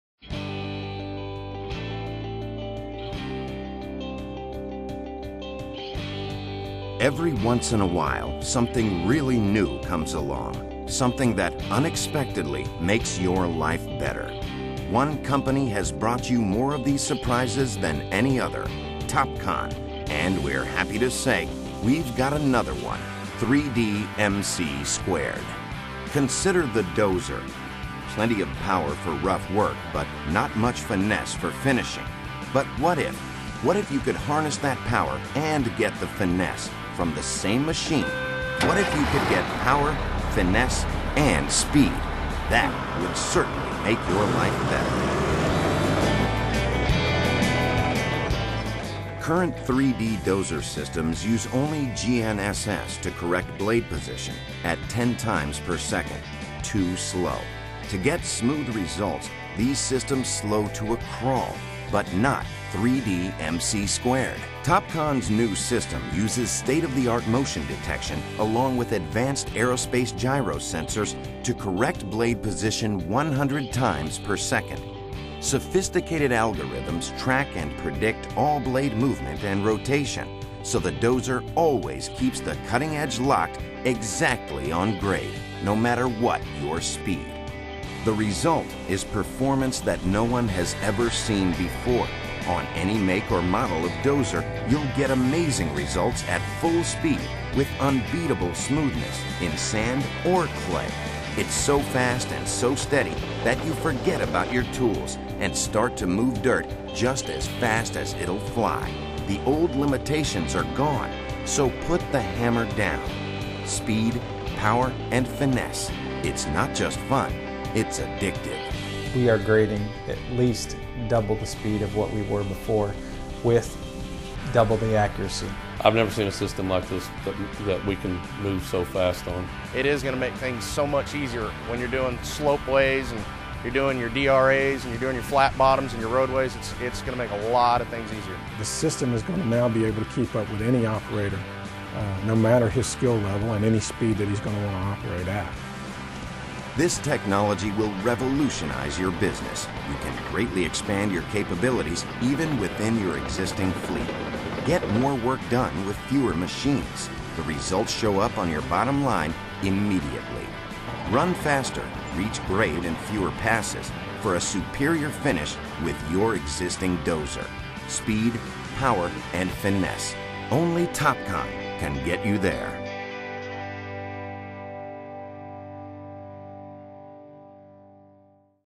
Topcon 3D-MC2
3D-MC2 will drastically change the role your dozer plays on a typical site, in some cases eliminating the need for a grader. It will also allow you to make more efficient use of your dozer. Along with speed increases, 3D-MC2 means fewer passes resulting in much more work done in less time. Less time equals less fuel and less machine wear. Most importantly, it means your dozer can spend more time making you money in another location or on other jobs.